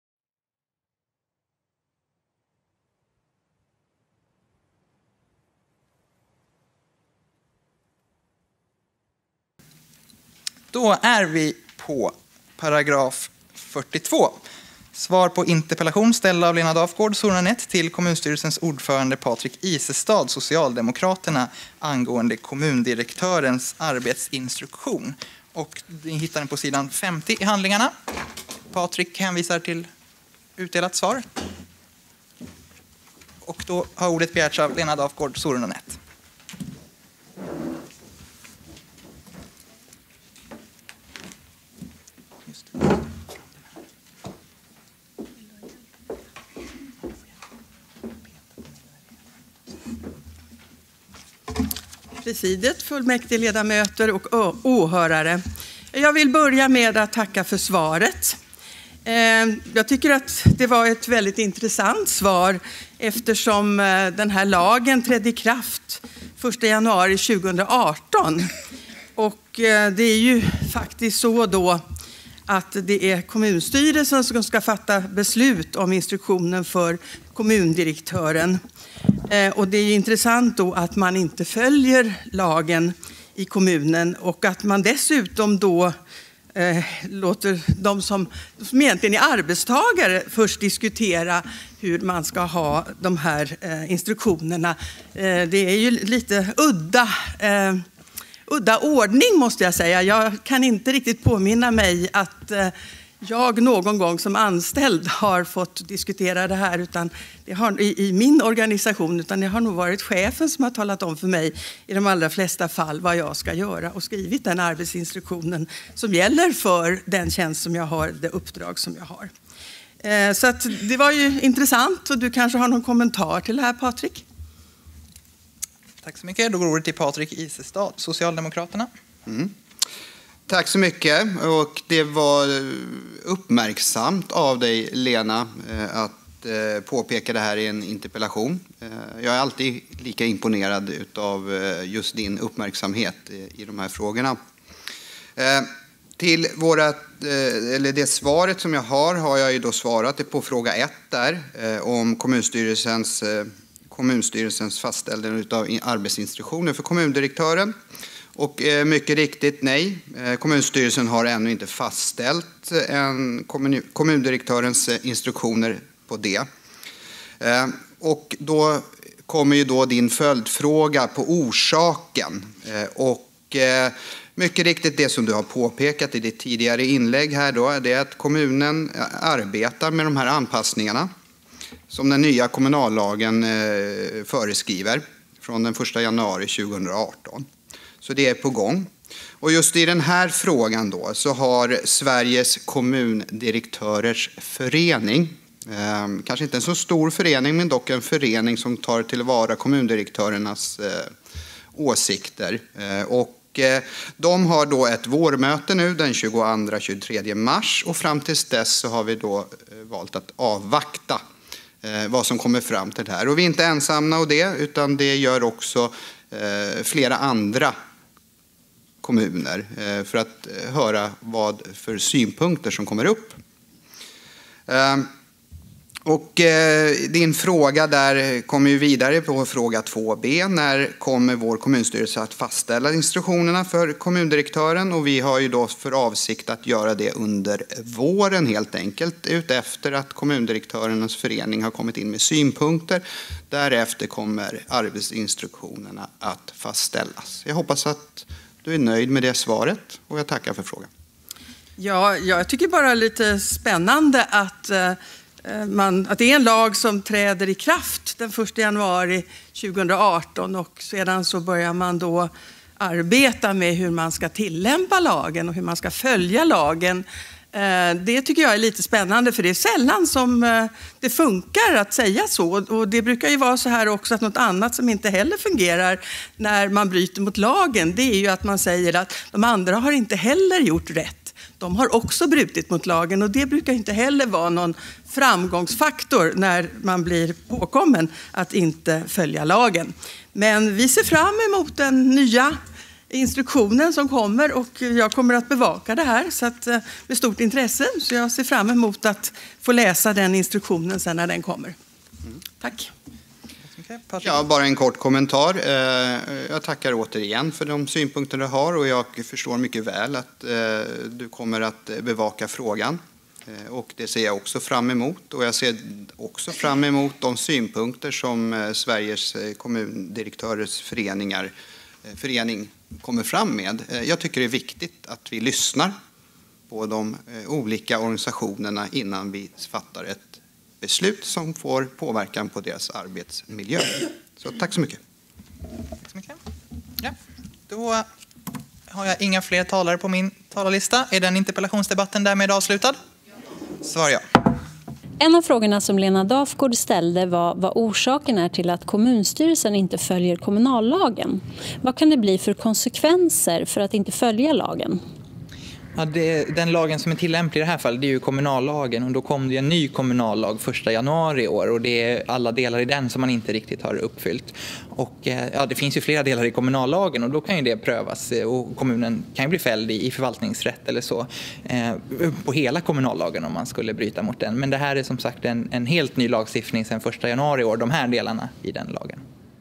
Nynäshamns kommunfullmäktige 20180314
§ 42 Svar på interpellation angående kommundirektörens arbetsinstruktion

Svar på interpellation ställd av Lena Dafgård (SN) till kommunstyrelsens ordförande Patrik Isestad (S), angående kommundirektörens arbetsinstruktion.

Lena Dafgård (SN) har den 15 januari 2018 inkommit med en interpellation ställd till kommunstyrelsens ordförande Patrik Isestad (S), angående kommundirektörens arbetsinstruktion.

Kommunstyrelsens ordförande Patrik Isestad (S) har besvarat interpellationen skriftligt.